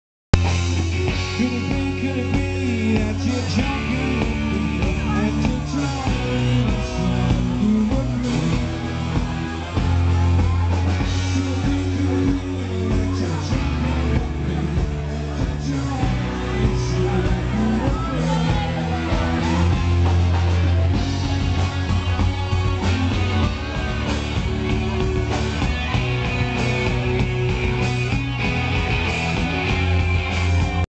wanderlust playing the hydro
this is wanderlust later when they repeated the song and the bar was full of a much lubricated crowd